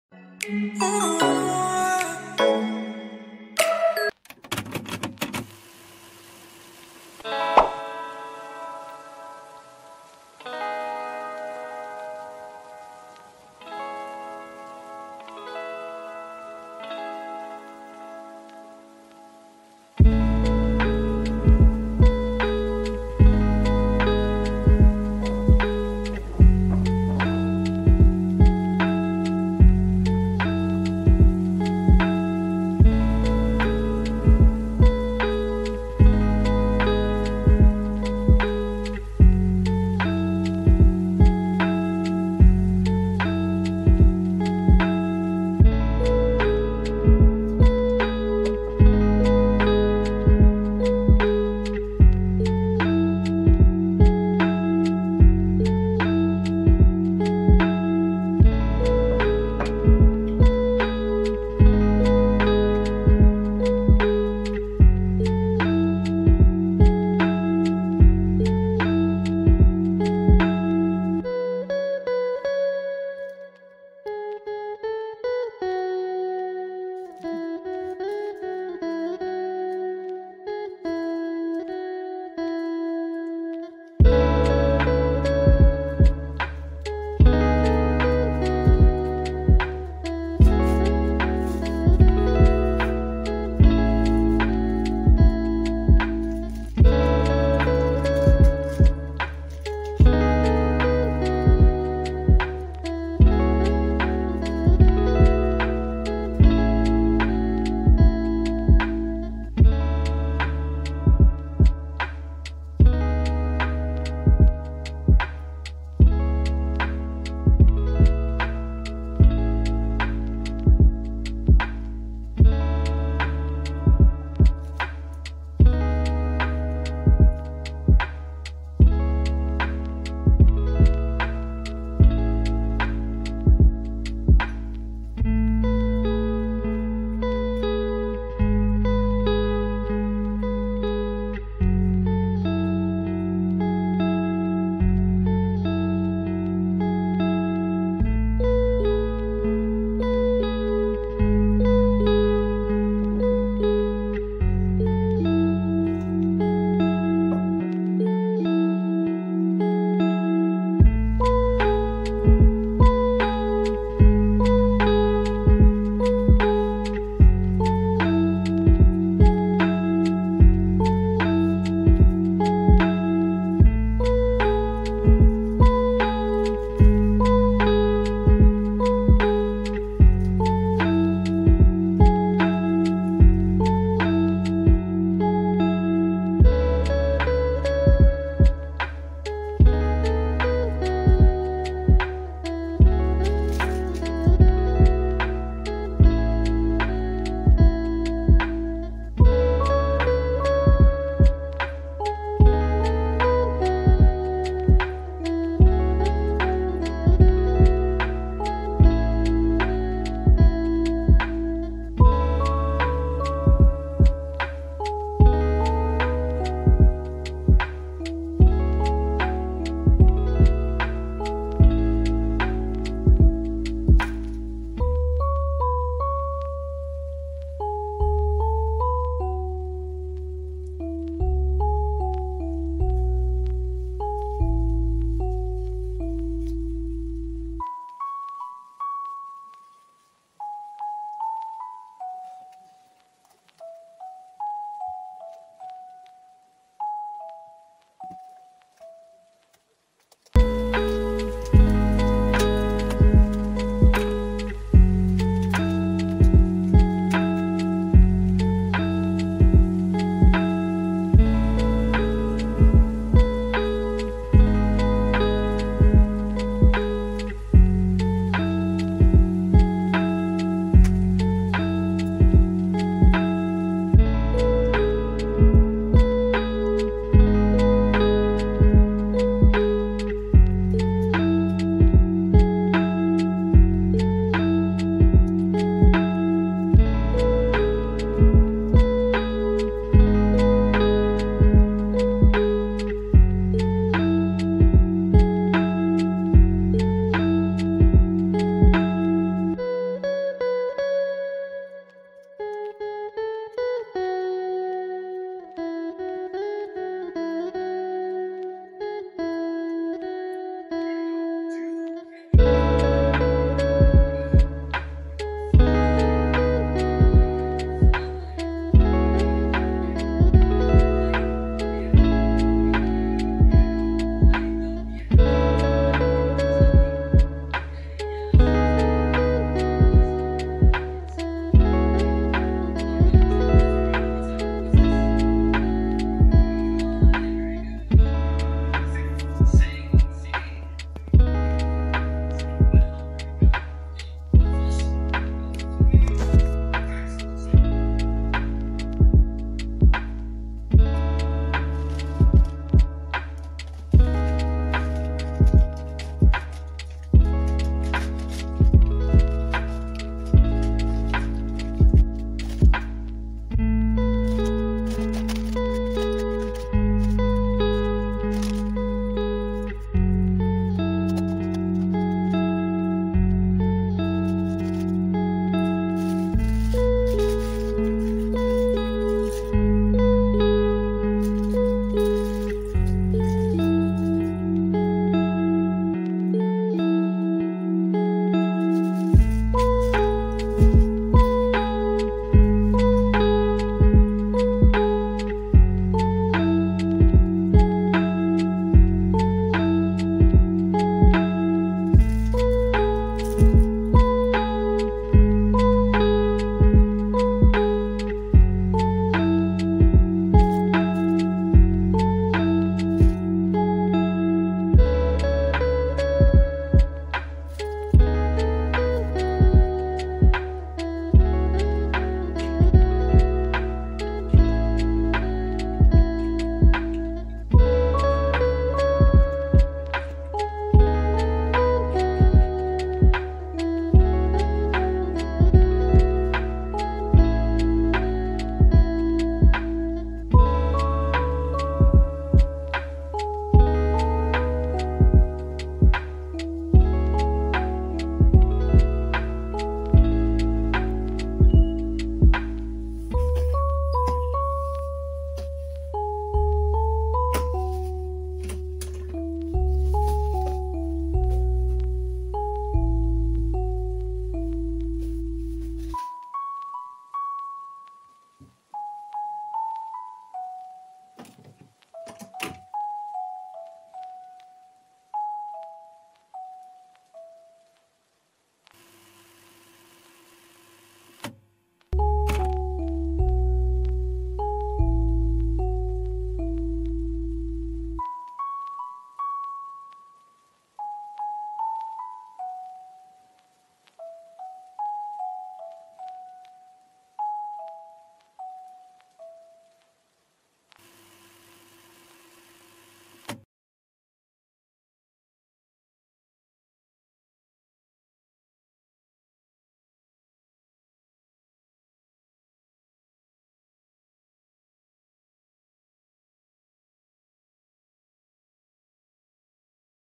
QUARANTINE IN JAPAN: Day in my life in a quarantine hotel
This is what a day in quarantine looked like for me.
I ate, attended my online orientation class for the day and that was it.